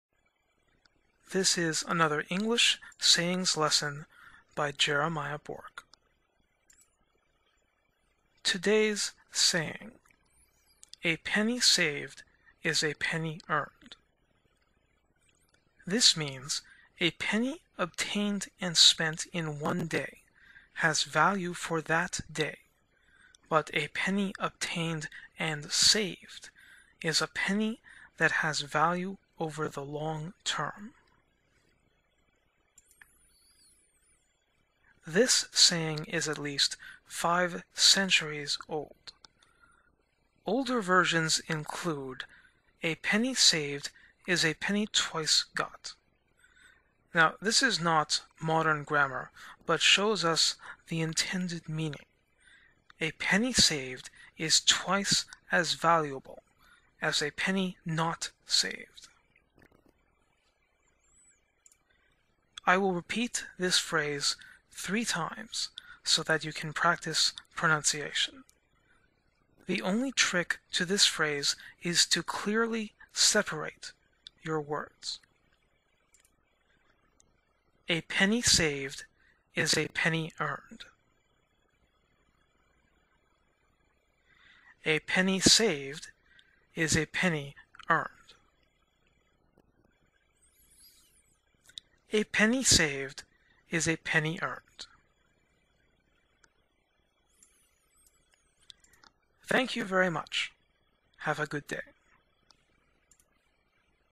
A Penny Saved Is A Penny Earned
An educational video using a common English saying. By Jeremiah Bourque